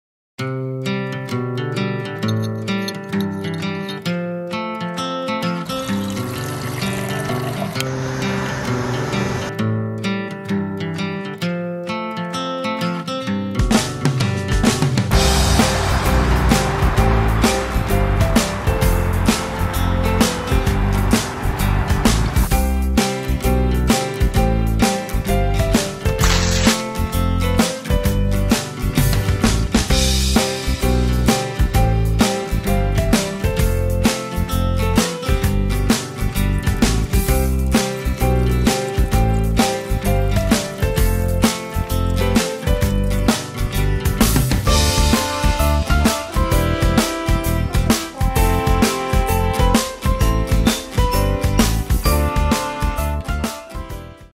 Can't Beet Chocolate Oatmeal
Needed a subject for a quick video so I decided to use this delicious oatmeal from Picky Bars.